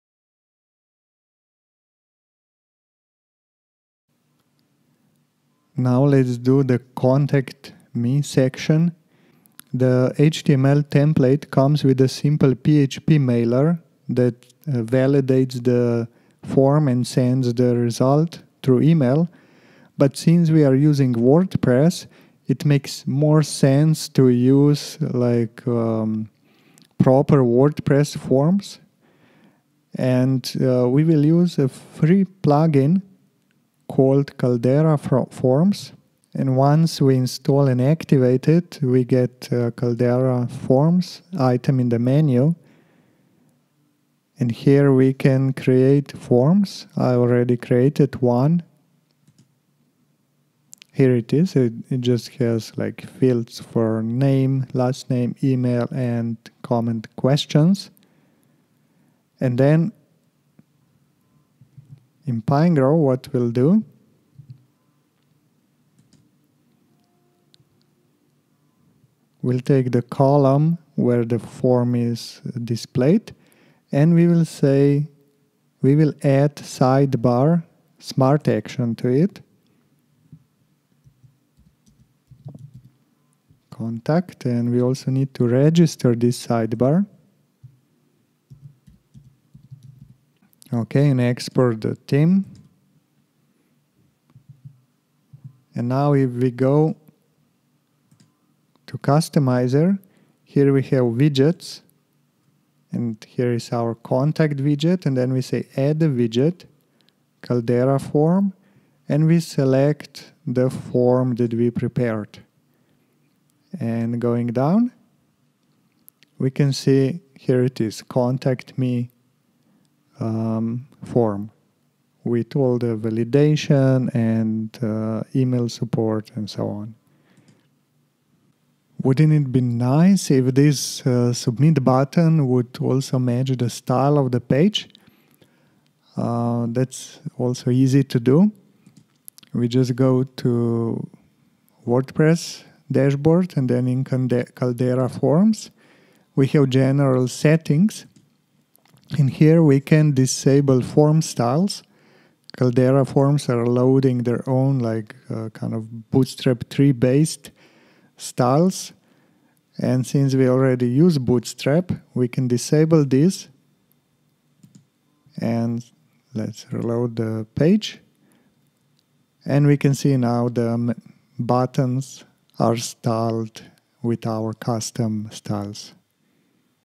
6. The Contact Form - Create a Portfolio WordPress Theme - Tutorial
We’ll use Caldera Forms WordPress plugin and a dynamic sidebar to display the form. Import Olaf’s form in Caldera Forms to save you the trouble of creating a new form.

This is a part of the tutorial about creating a WordPress theme with Pinegrow, without PHP coding